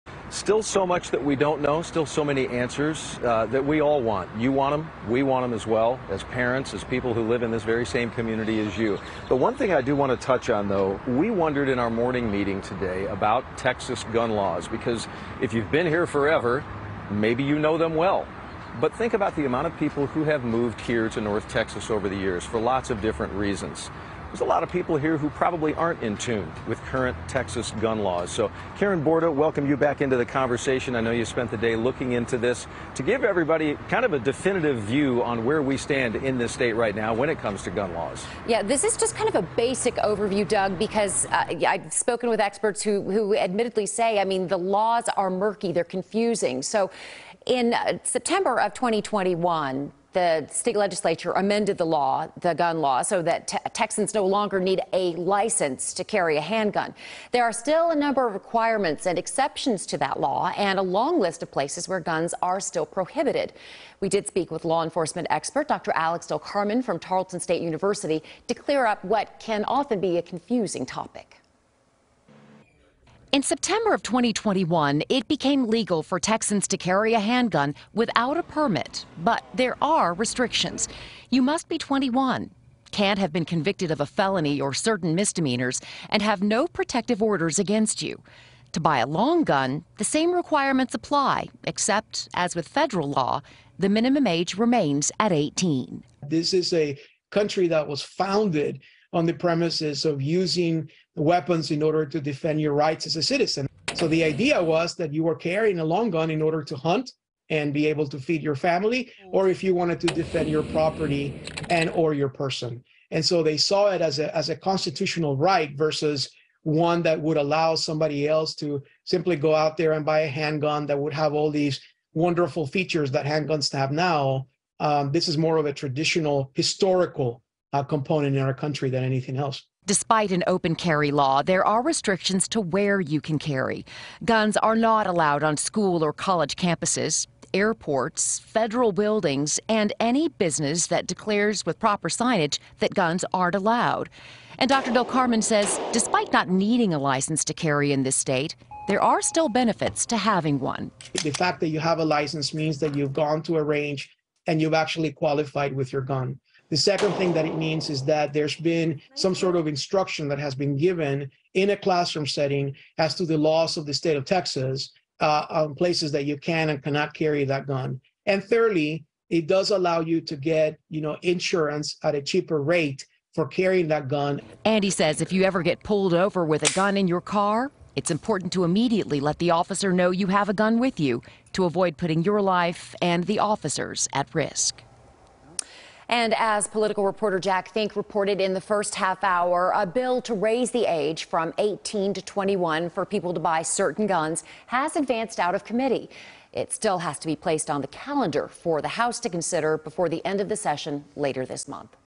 Who can carry a gun in Texas?
CBS News Texas decided to look into what Texas' laws say about who can own firearms and where it's legal to carry them.

In the wake of the shooting at an Allen outlet mall that left eight victims dead, there are still a lot of questions lingering about the gunman and whether he legally owned his weapons. CBS News Texas decided to look into what Texas' laws say about who can own firearms and where it's legal to carry them.

Two years ago, the Texas Legislature amended state law so that Texans no longer need a license to carry a handgun. But there are still a number of requirements and exceptions to the law -- and a long list of places where guns are still prohibited.

To help clear up some of the confusion, CBS News Texas anchor Karen Borta spoke with a law enforcement expert.